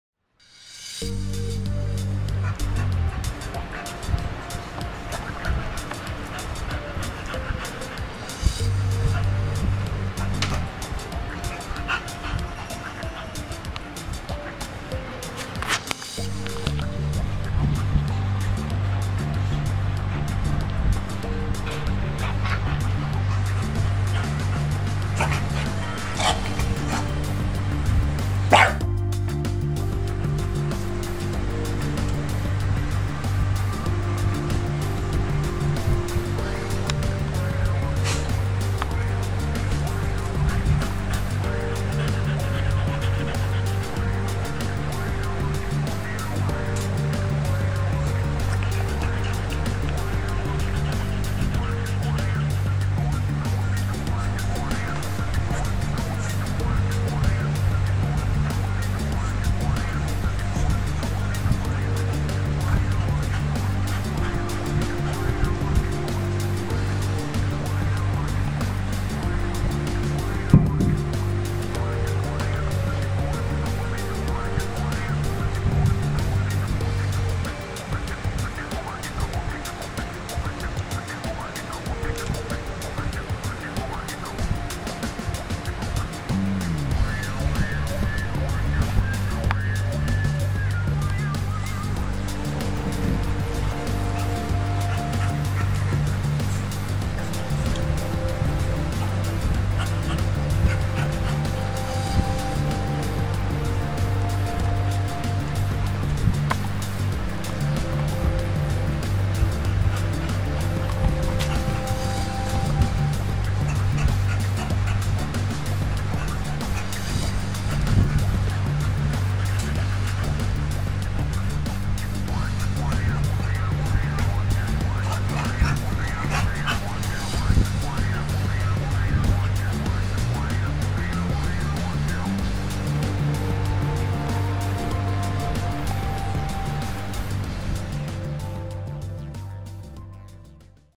Molly Molester
Molly wants to get a groove on, lucky for her Mr. Mickey is cool enough to allow it.